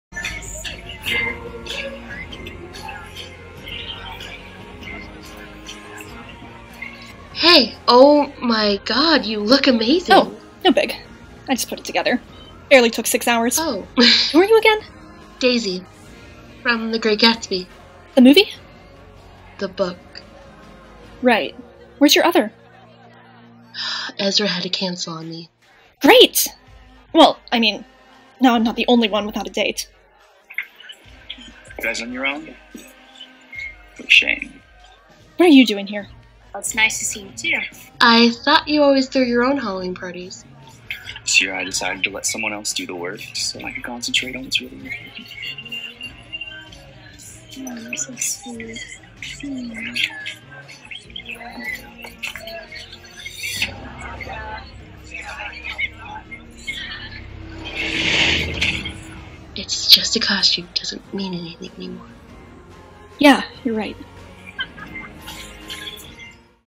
pll voiceover collab | "barely took six hours" w/ Haley Radiant
I'll always love collabing with the talented Haley! This scene turned out too well!!

Aria - Haley Radiant
Hanna - me